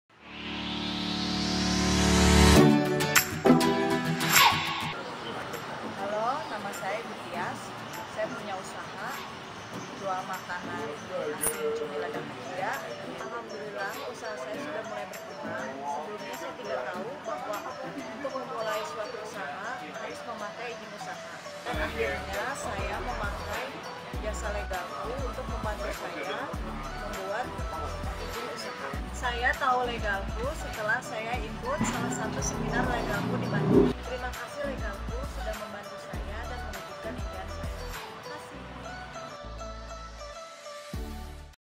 Testimonial Legalku - Perizinan Usaha
Ibu Tyas adalah seorang pendagang yang menjual makanan asinan cumi pedas untuk lauk makan. Beliau mulai usahanya dari kecil-kecilan secara online hingga sekarang mempunyai kedai sendiri dengan berbagai macam menu yang dijual di kedainya. Ibu Tyas ini ingin melegalkan usaha kedainya melalui pengurusan izin usaha. Mengetahui Legalku dari workshop Legalku yang pernah diadakan di Bandung, beliau menghubungi Legalku untuk membantu dalam pengurusan izin usaha.